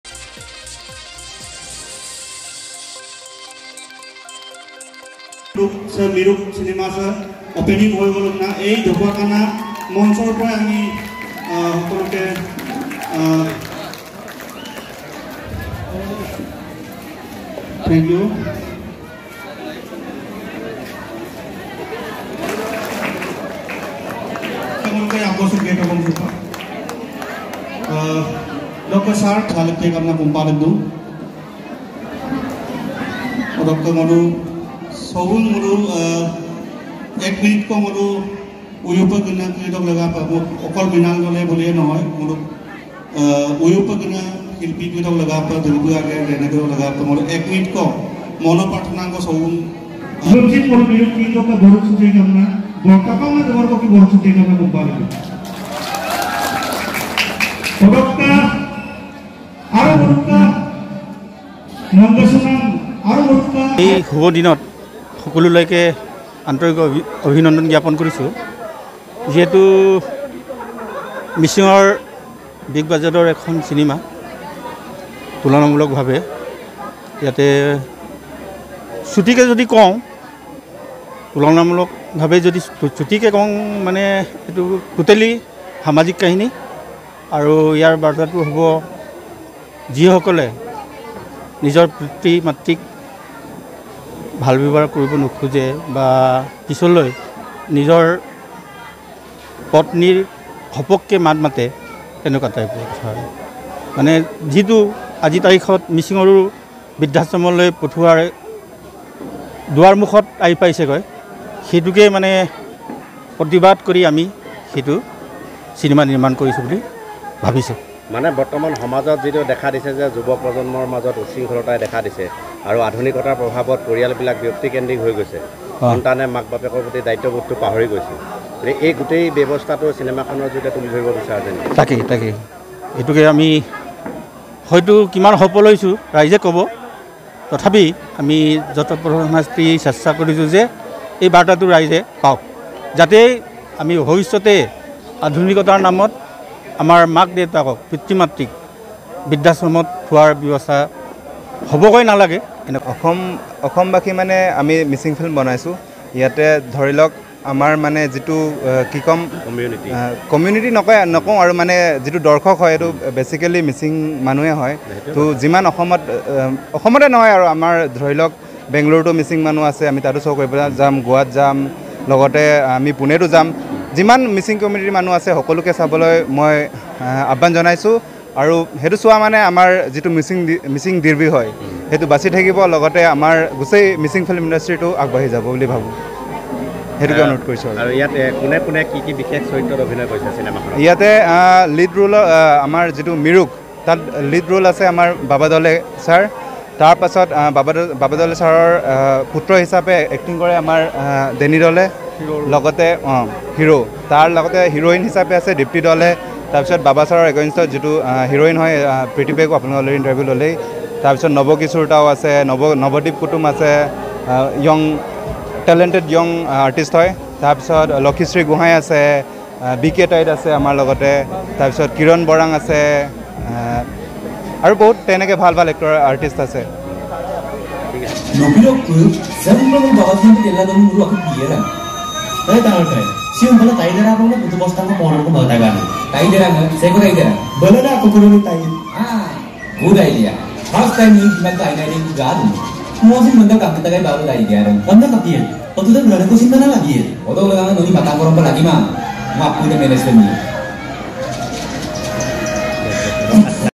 News:-ঢকুৱাখনাত মিচিং চিনেমা-MIRUG-ৰ প্ৰিমিয়াৰ শ্ব সম্পন্ন || Mising Film MIRUG Premiere Today
@ঢকুৱাখনাত মিচিং চিনেমা-MIRUG-ৰ প্ৰিমিয়াৰ শ্ব সম্পন্ন।